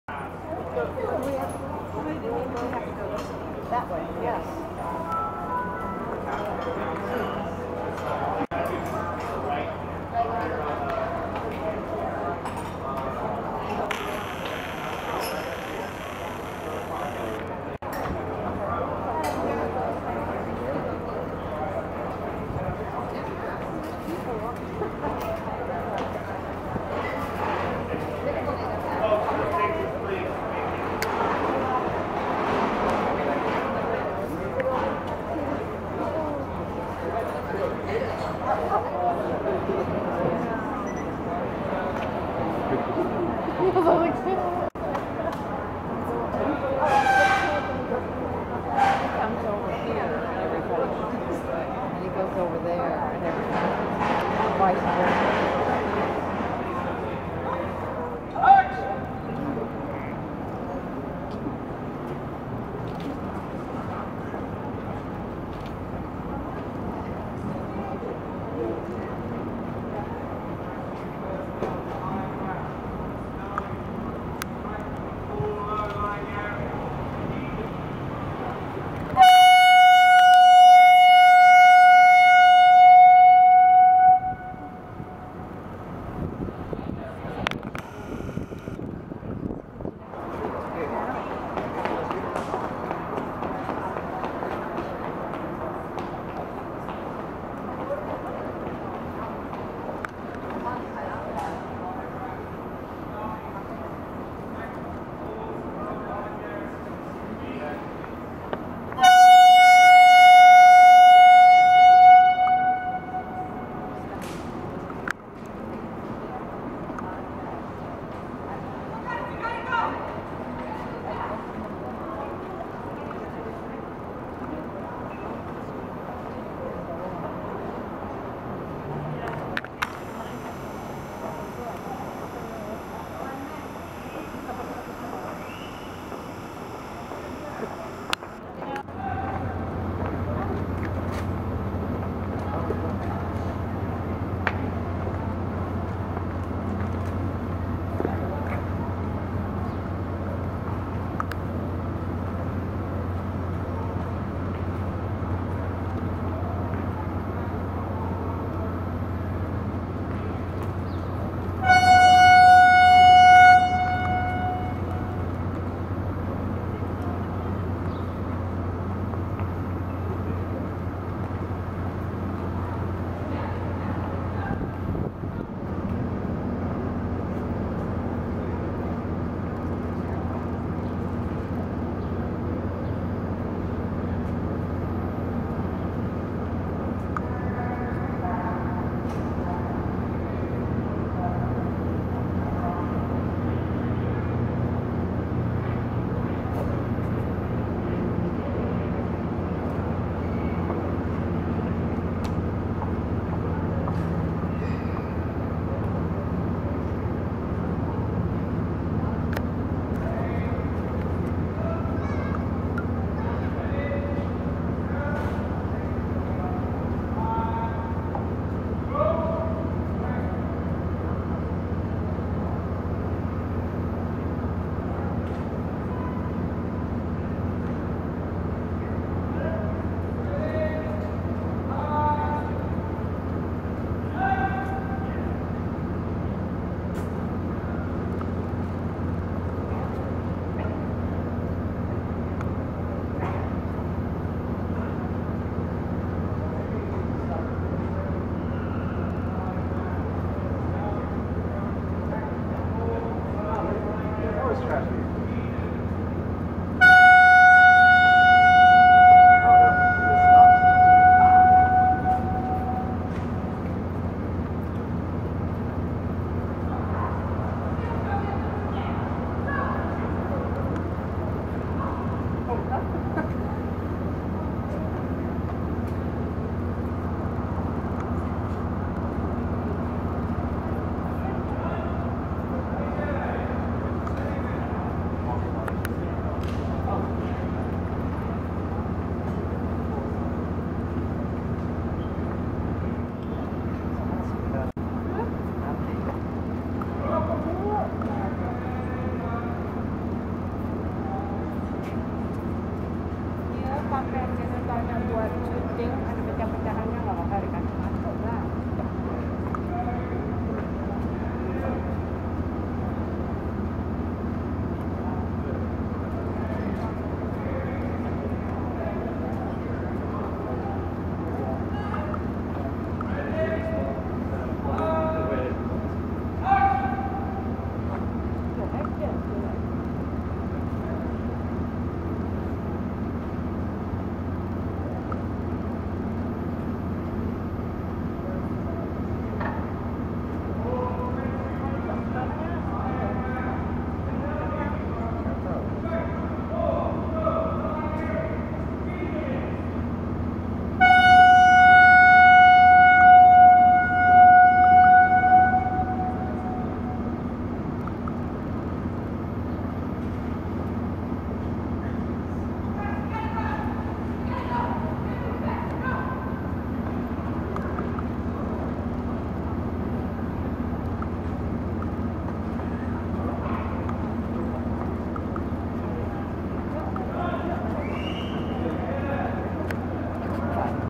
"San Andreas" Film Shoot @ Chinatown Gate Bush St & Grant Ave San Francisco California 2014
"San Andreas" film shoot near the Chinatown Gate on Bush Street and Grant Avenue in downtown San Francisco on Sunday, July 27, 2014.